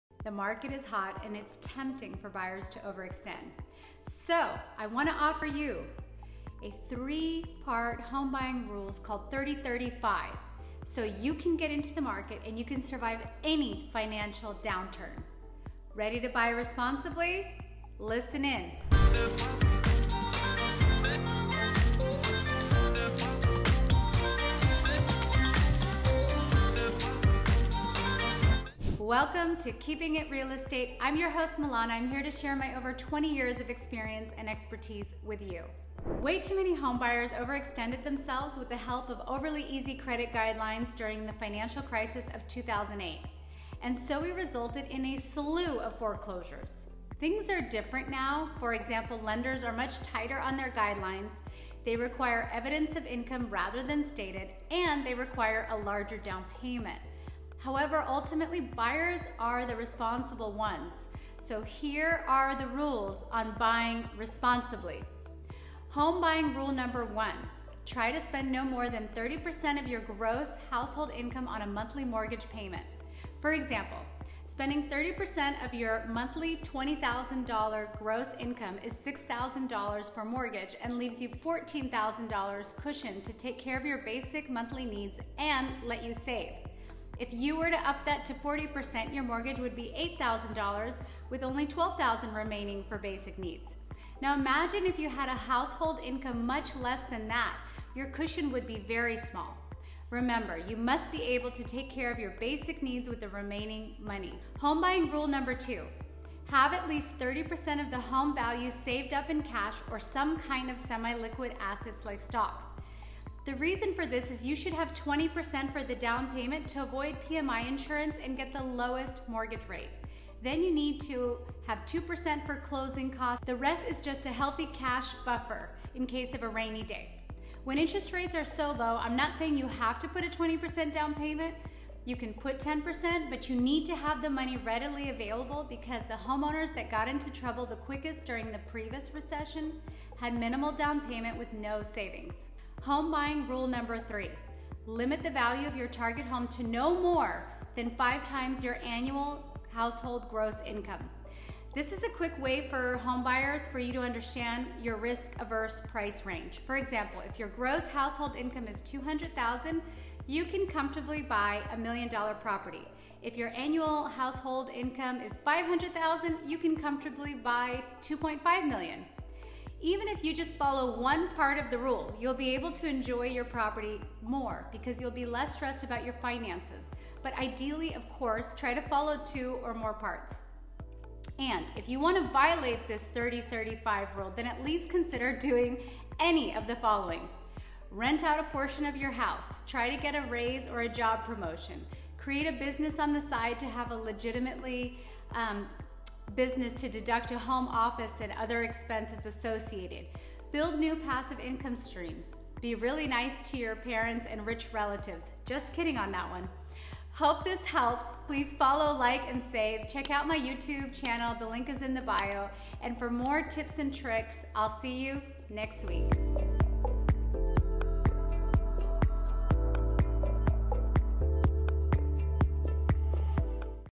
Home Buying Rules to Survive any Recession | Milana Real estate Group
The market is hot and it's tempting for buyers to overextend. So I want to offer you, risk-averse home buyers. There are three home-buying rules called 30/30/5 so you can get into the market and you can survive any financial downturn. Ready to buy responsibly? So, let's watch entire the video and learn about home-buying rules to survive any recession and tips to buy a home in a down market.

Way too many homebuyers overextended themselves with the help of overly easy credit guidelines during the financial crisis of 2008 and which resulted in a slew of foreclosures. Things are different now, for example, lenders are much tighter on their guidelines, they require evidence of income rather than stated and they require a larger down payment. However, ultimately buyers are responsible so the rule on buying responsibly…

Home Buying Rule #1: Try to Spend no more than 30% of your gross household income on a monthly mortgage payment.

Home Buying Rule #2: Have at least 30% of the home value saved up in cash or some kind of semi-liquid asset like stocks.

Home Buying Rule #3- Limit the value of your target home to no more than 5 times your annual household gross income.

Even if you follow just one part of the rule, you will be able to enjoy your property more because you will be less stressed about your finances. But ideally of course follow 2 or more parts! And if you want to violate the rule, then at least consider doing any of the following: Rent out a portion of your house, try to get a raise or job promotion, create a business on the side to have a way to legitimately deduct a home office and other expenses, build new passive income streams and be really nice to your parents and rich relatives, just kidding on that last one.

If this video was helpful, please do subscribe, like, comment, and share; if you have any real estate-related questions or you're thinking of selling your house, contact the best realtor Milana Ostroy.